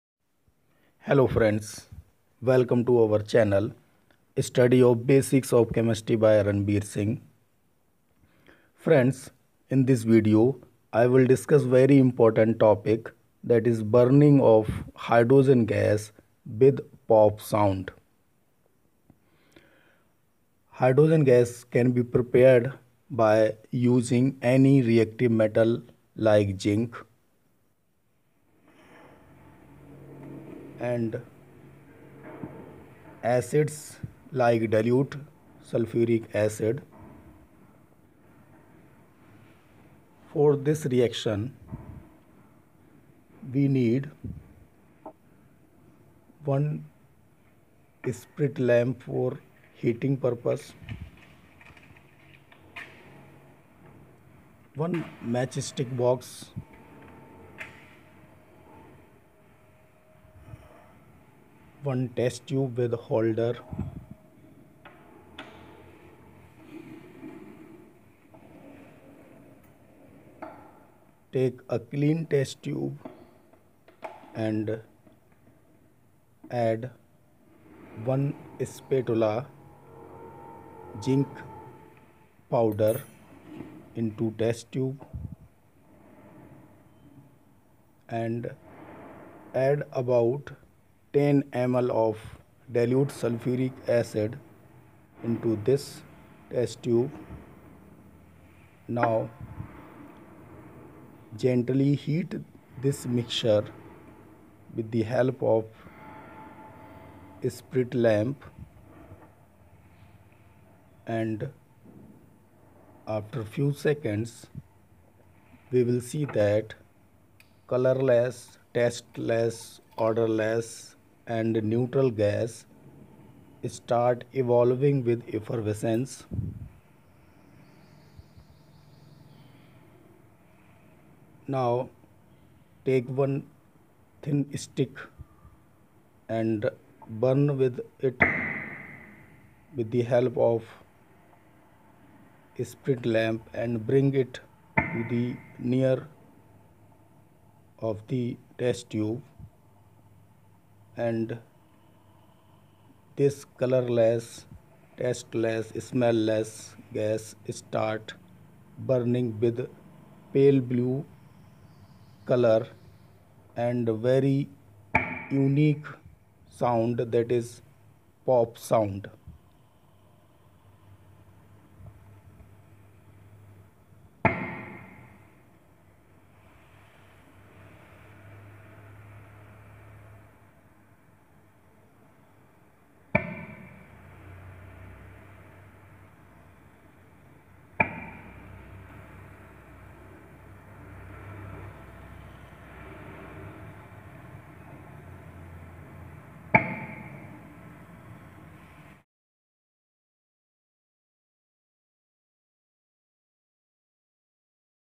popsoundPreparation (in lab.), properties and identification of Hydrogen gas.It's Combustion.
Preparation of Hydrogen gas with the help of zinc metal and dil sulphuric acid and it's burning with stick in chemistry lab. Hydrogen gas is combustible in nature and burn with pop sound, it is lightest,colourless, testless, neutral gas and released with effervescence.